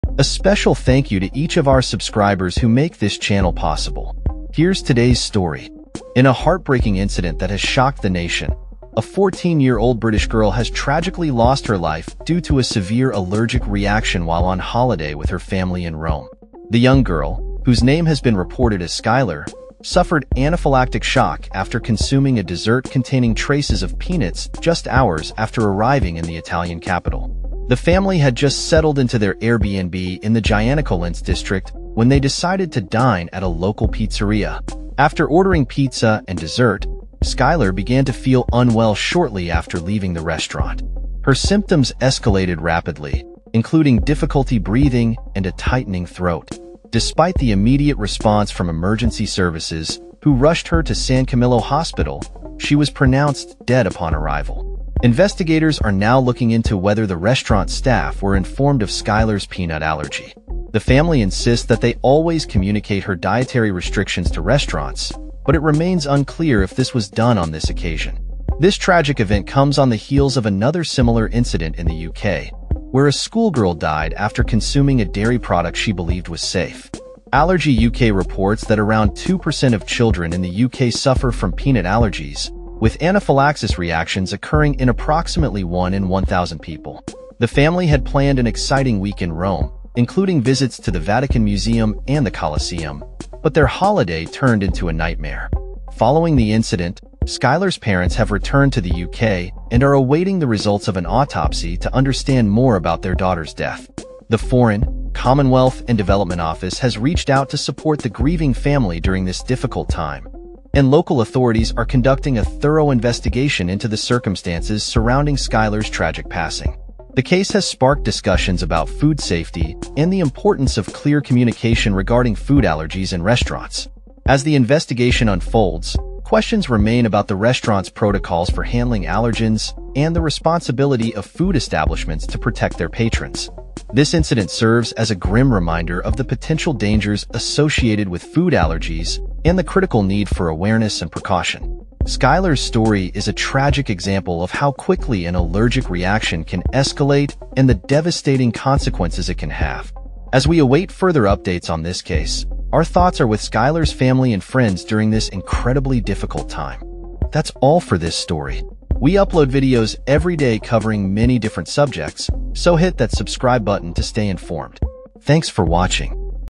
Tragic Vacation: British Teen Dies from Peanut Allergy in Rome Restaurant
In a heartbreaking incident that has shocked the nation, a 14-year-old British girl has tragically lost her life due to a severe allergic reaction while on holiday with her family in Rome. The young girl, whose name has been reported as Skylar, s ...